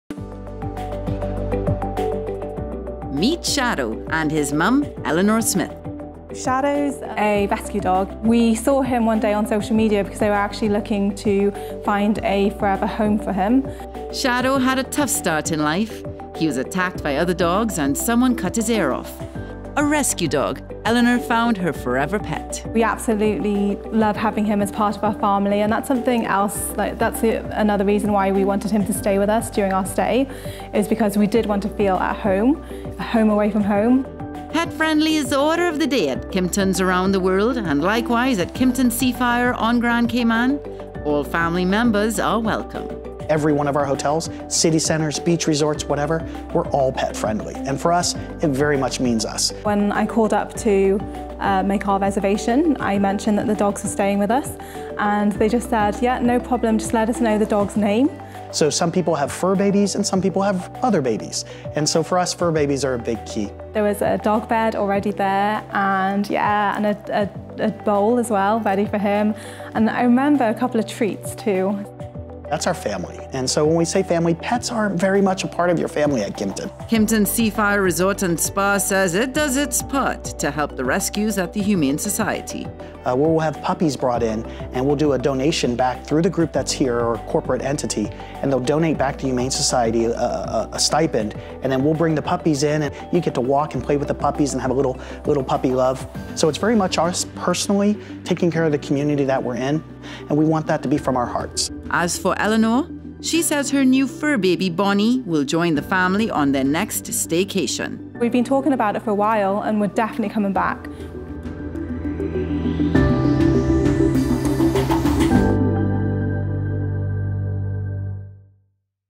Love your pets, Kimpton Seafire Resort + Spa welcomes all family members
At Kimpton Seafire Resort + Spa, pets are welcomed like family members. One recent guest at the resort says having her rescue dog stay with them made their Staycation even better.